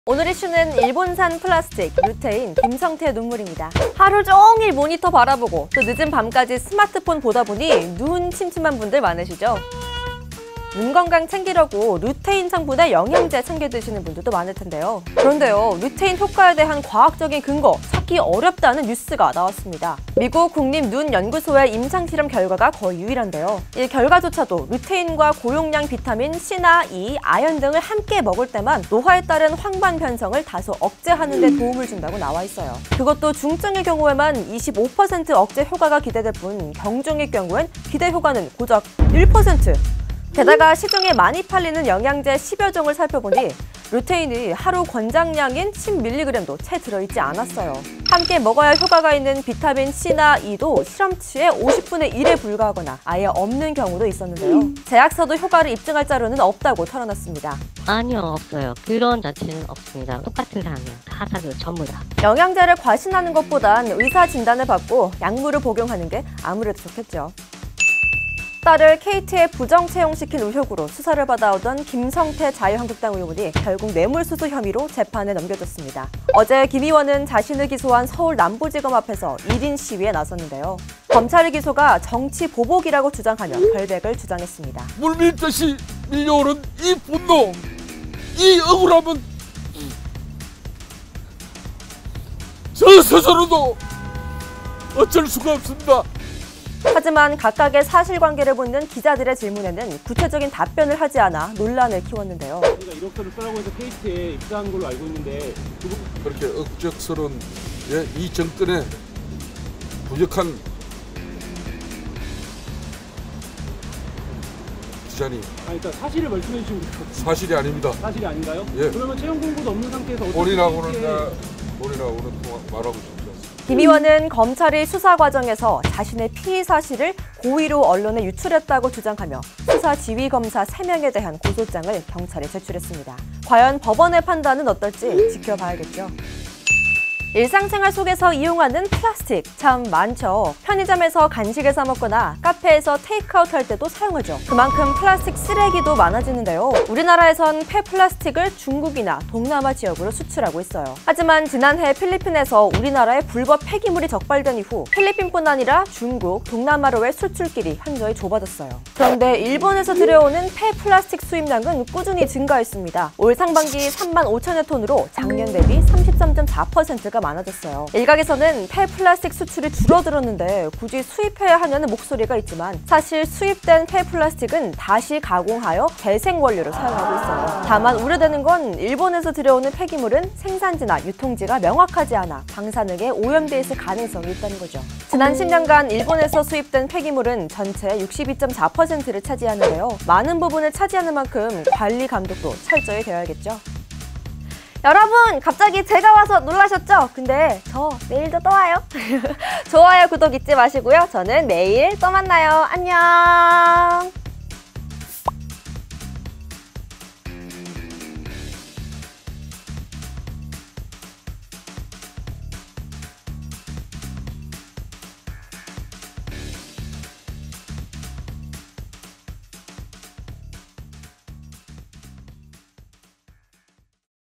'눈 좋아진다'는 루테인. 사실상 효과는 미미하다 / 14F
눈 건강을 위해 루테인 성분의 영양제 챙겨먹는 분들 많으시죠. 그런데 그 효과에 대한 과학적 근거는 찾기 어렵다고 합니다. 루테인과 고용량 비타민 C나 E, 아연 등을 함께 먹을 때에나 중증 이상의 ‘노화에 따른 황반변성’을 다소 억제하는데 도움을 준다고 하는데요. 게다가 시중 판매 제품 중 하루 권장량도 채 들어있지 않은 제품도 있었습니다.